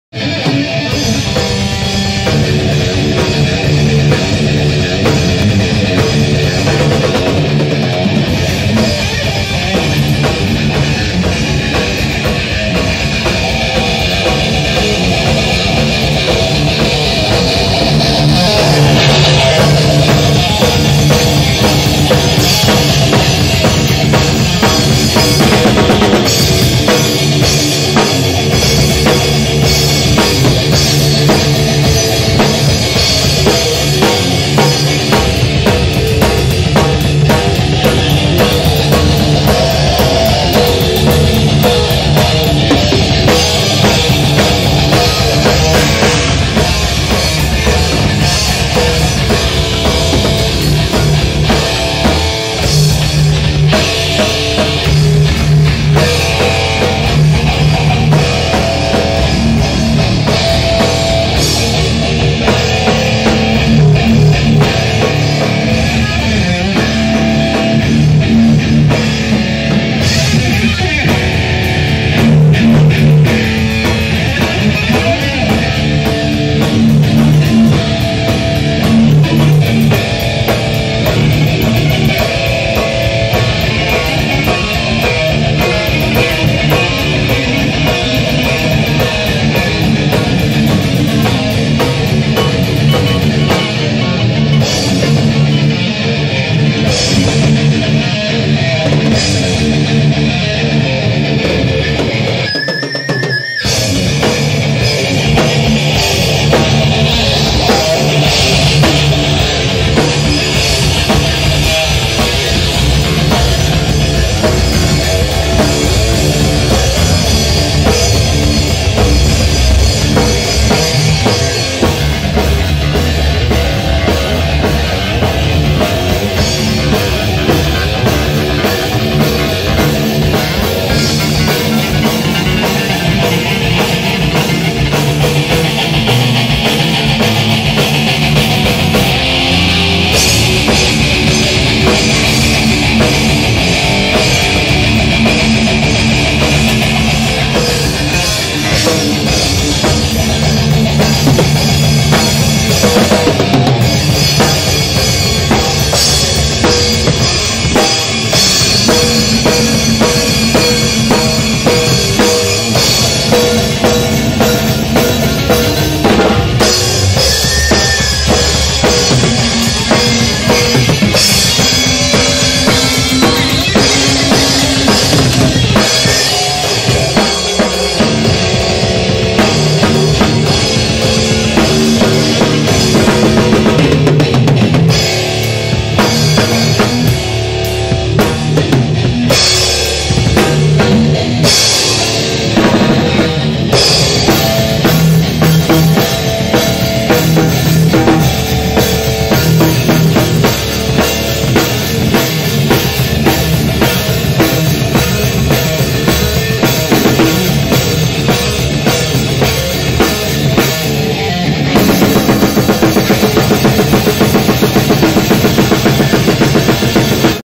Driven by Rage
The band Driven by Rage rehearsing at AYRME Studios.